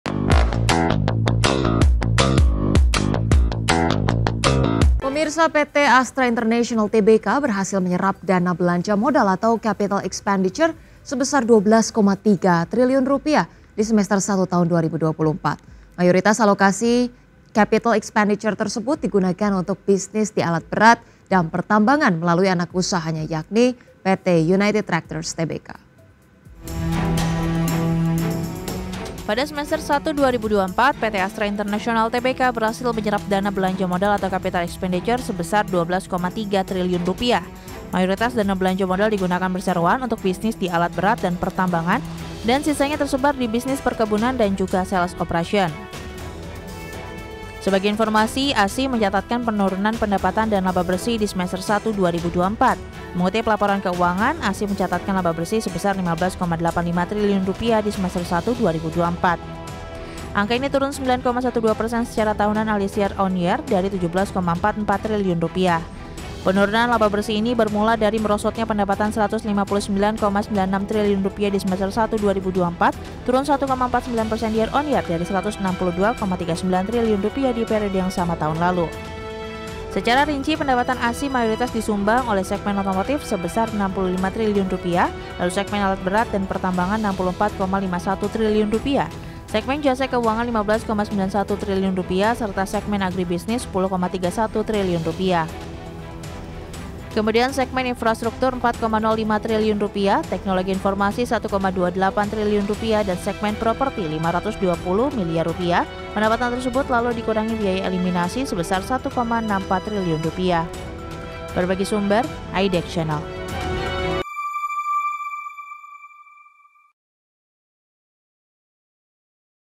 Penggunaan Dana Capex ASII | IDX CHANNEL
PT Astra International Tbk (ASII) berhasil menyerap dana belanja modal atau Capital Expenditure sebesar Rp12,3 triliun di Semester I-2024. Mayoritas alokasi Capital Expenditure tersebut digunakan untuk bisnis di alat berat dan pertambangan melalui anak usahanya yakni PT United Tractors Tbk (UNTR).